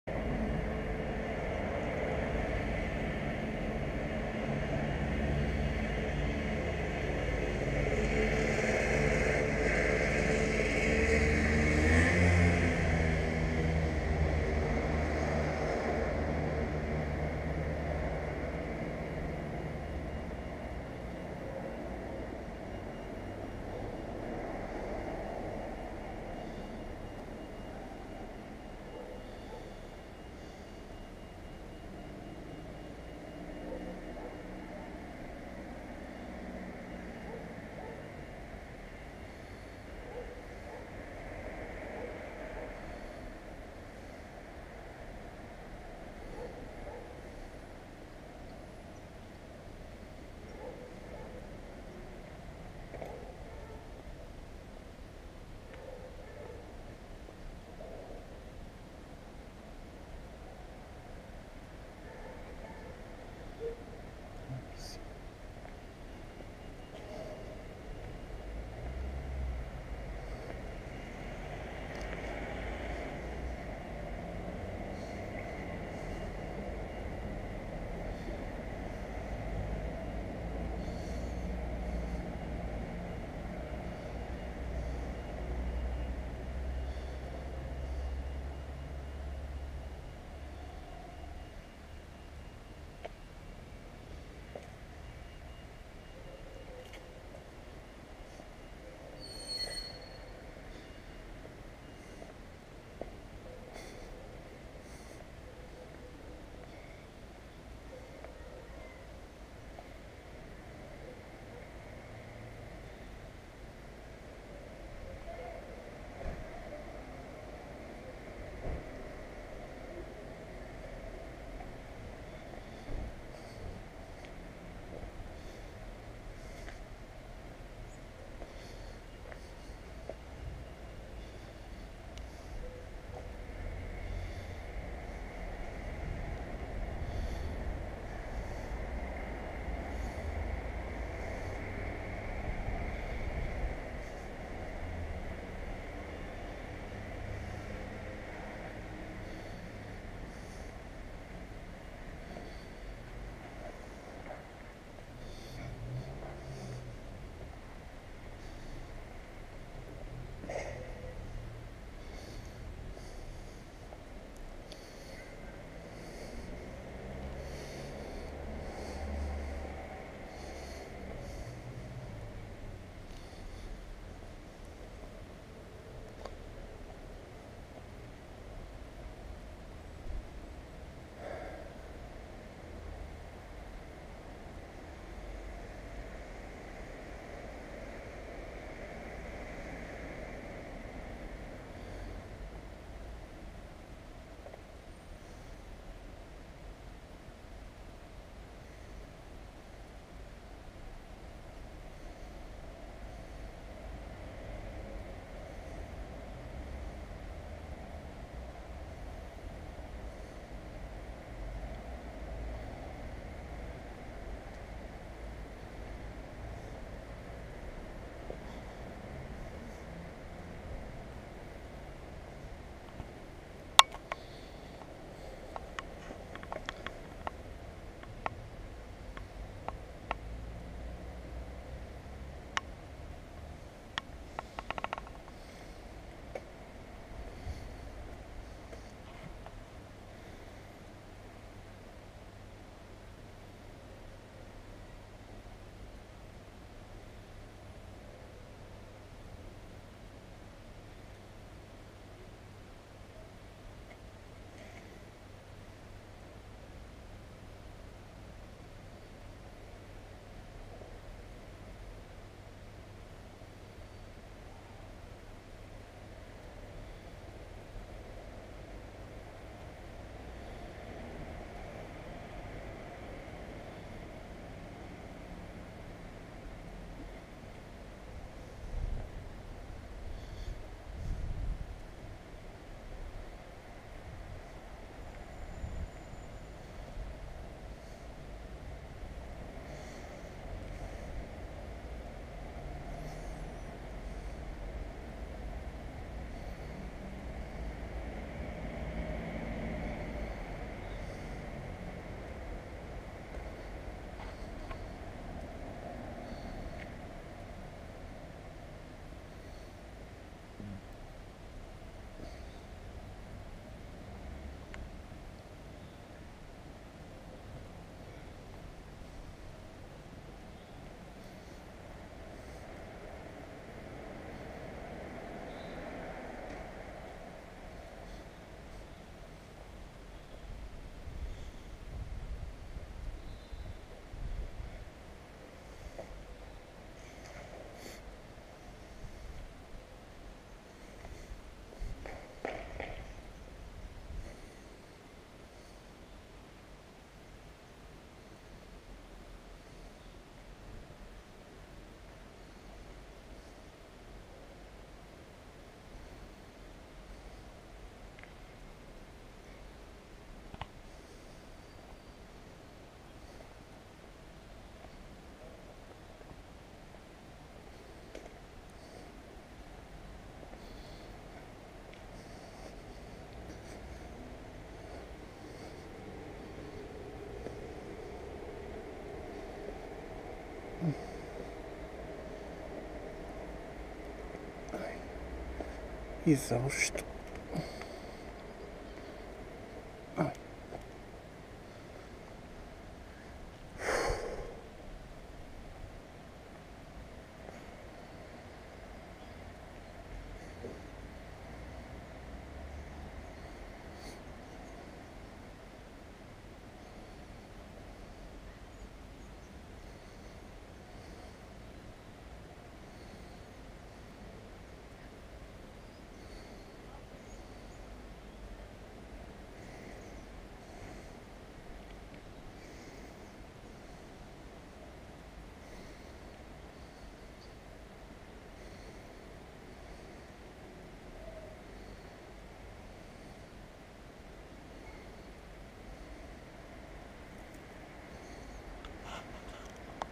Alguma beleza na Rua do Impasse - Some beauty on Rua do Impasse
PT - O local de descanso foi no fim desta gravação.

Video 11 de 16, gravado com uma GoPro Hero, em 720p 50fps.

EN - The resting place was on the end of this recording.

Video 11 of 16, recorded whit a GoPro Hero, in 720p 50fps.